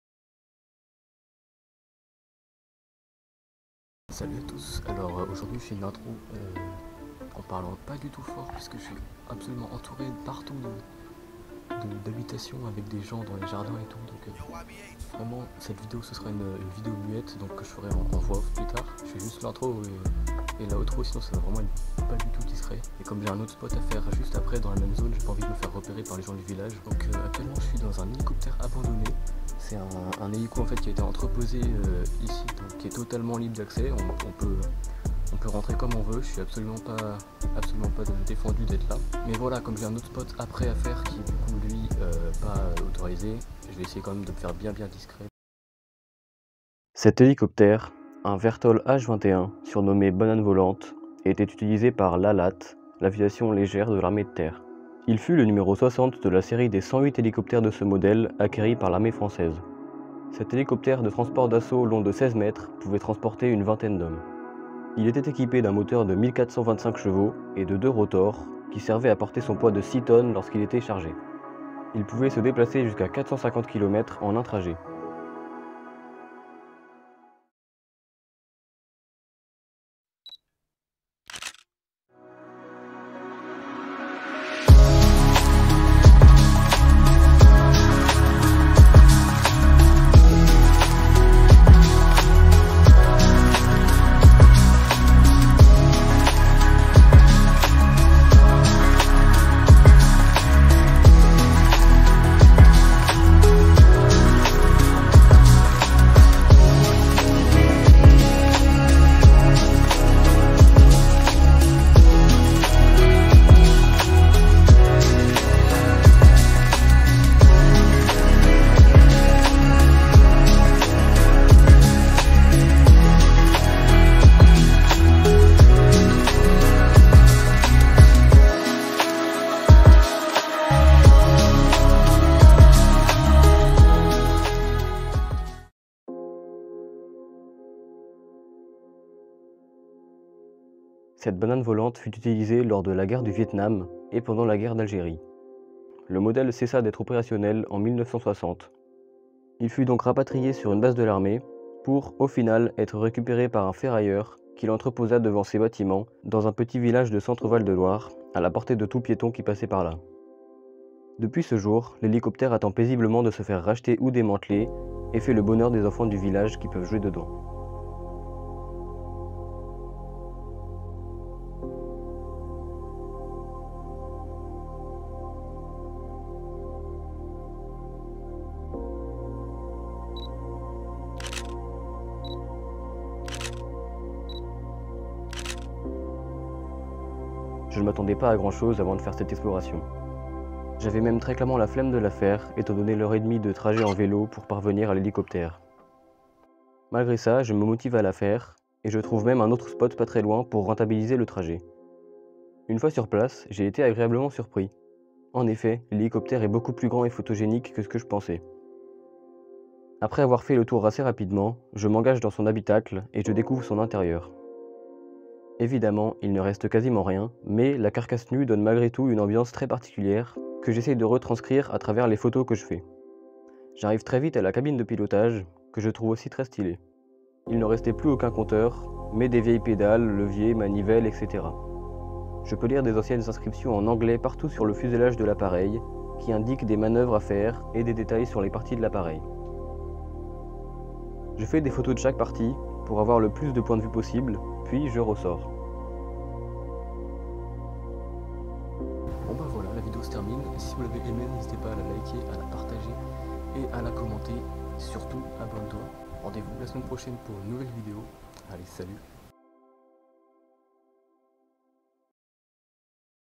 Je trouve un HÉLICOPTÈRE abandonné
Dans cette vidéo, pour ce nouvel urbex, j'explore un hélicoptère abandonné, qui était bien stylé et photogénique.

Insta perso: @tmt_dsh
Insta urbex: @french.urbexer